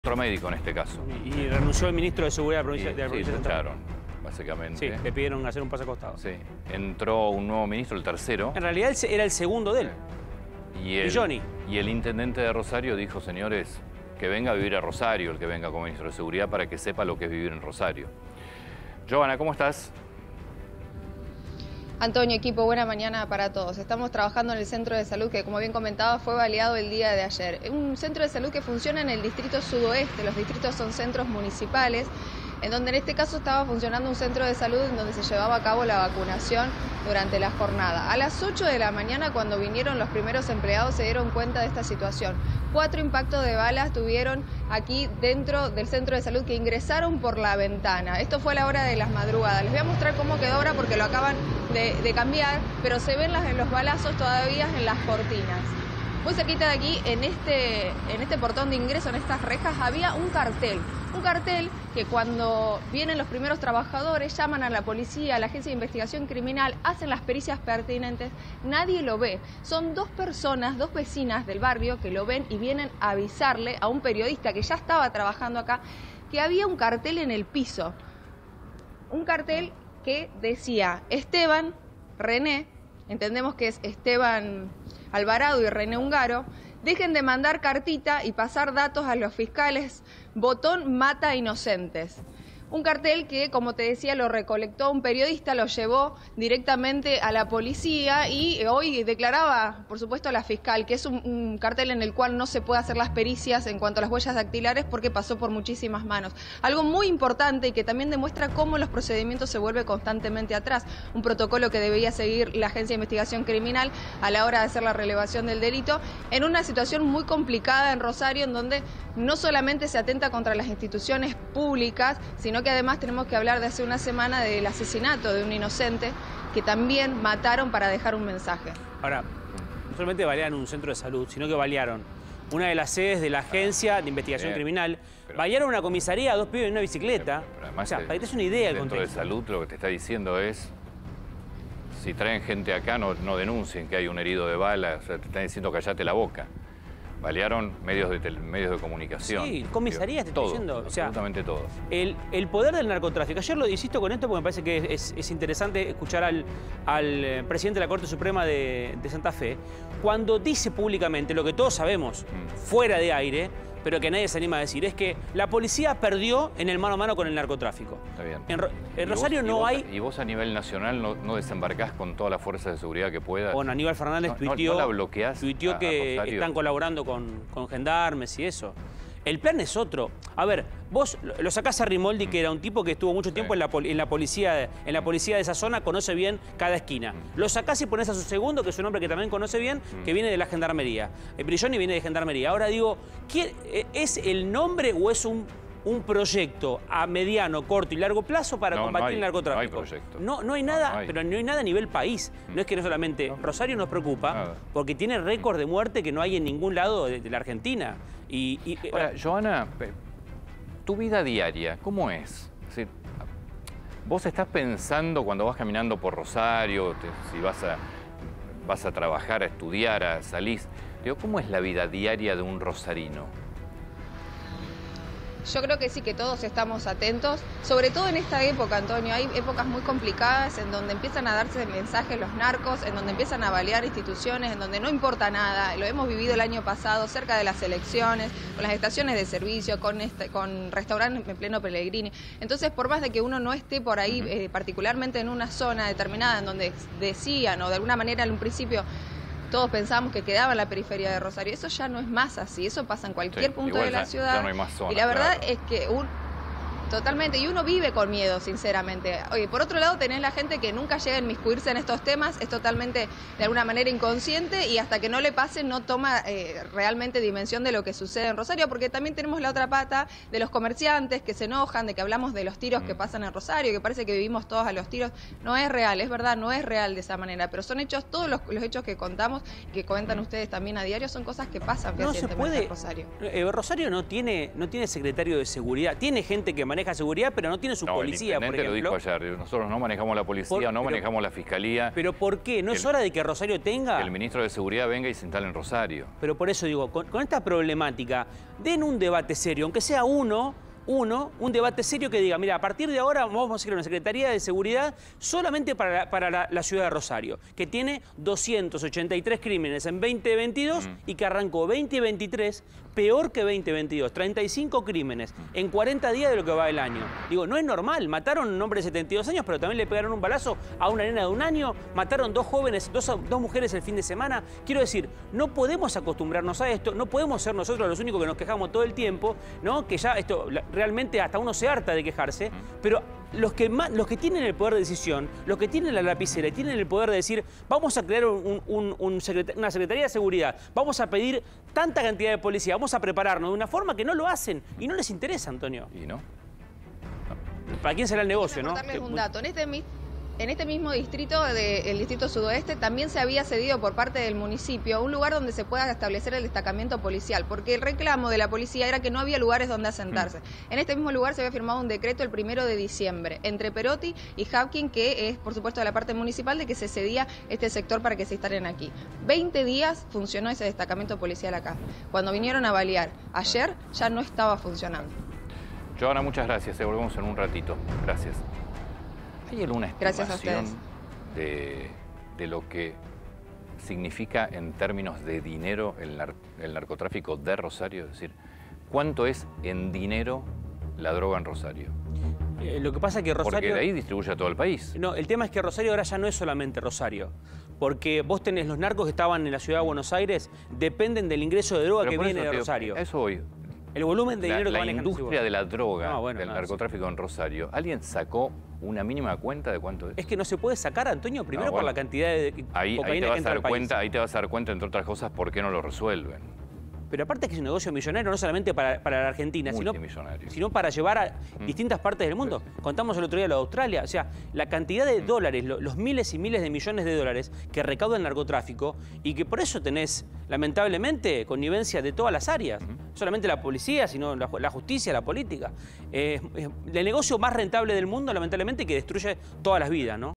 Sicarios sin límite en Rosario: balearon un centro de salud
Móvil en vivo desde Rosario.